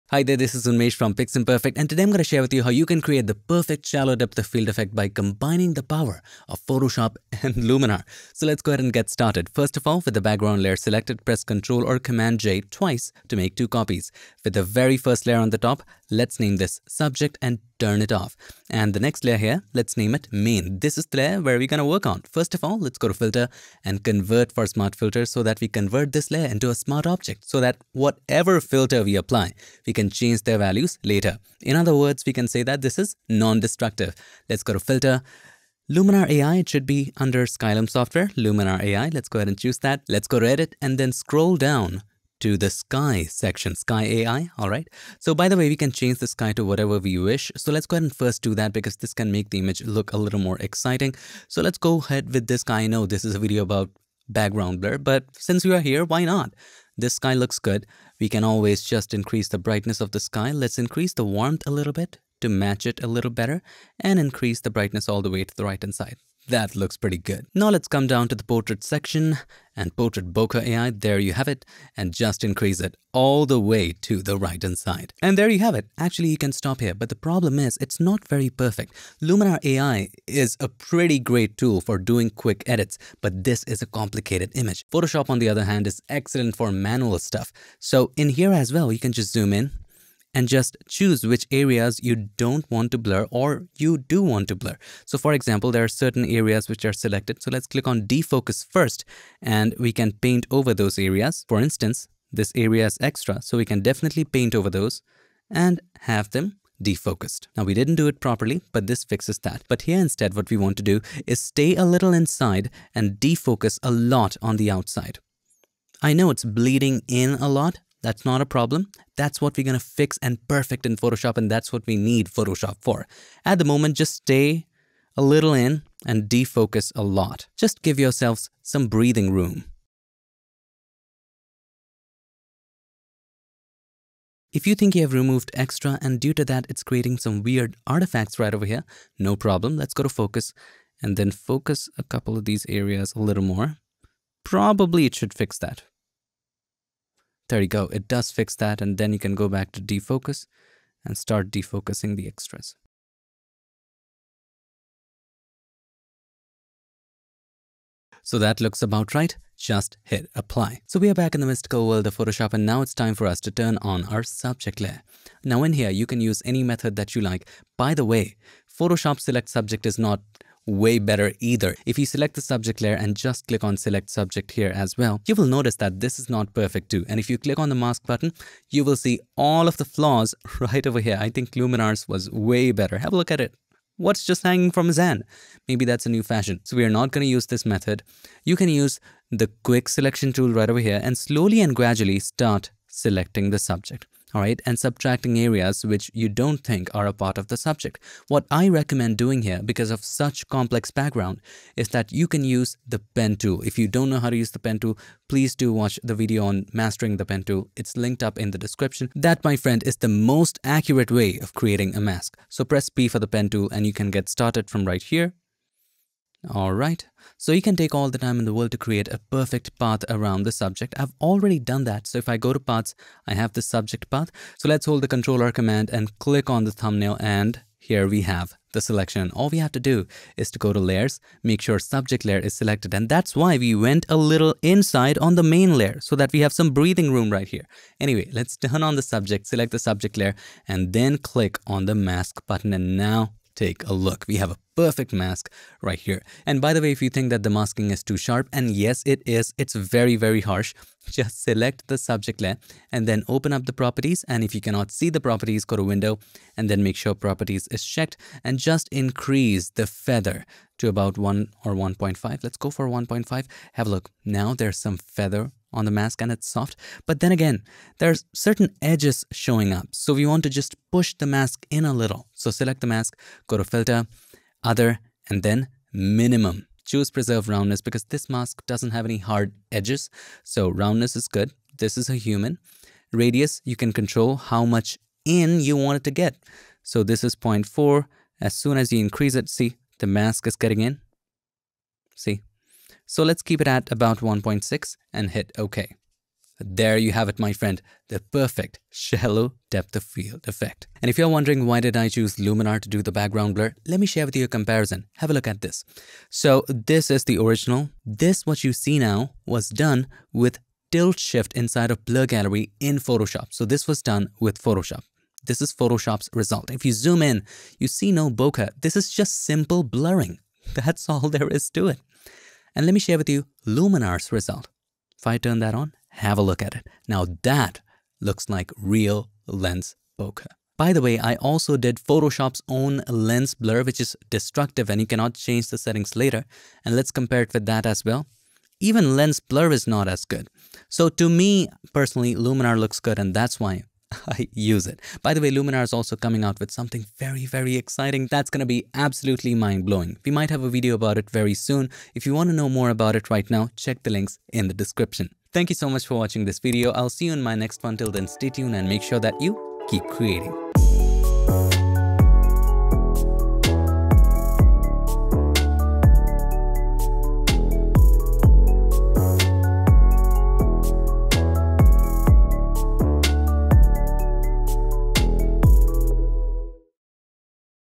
Photoshop + Luminar AI = PERFECT Background Blur!
Achieve the Perfect Shallow Depth-of-Field Effect with the Combination of Photoshop and Luminar AI. In this tutorial, we will combine the amazing power of artificial intelligence in Luminar to create Bokeh, with the unlimited manual controls in Photoshop to achieve the most natural background blur without any flaws.

► TIMESTAMPS:
00:00 Preparing for Edit
00:33 Process in Luminar
02:40 Process in Photoshop
05:00 Comparison of Photoshop & Luminar AI for Background Blur
05:44 Luminar's New Release?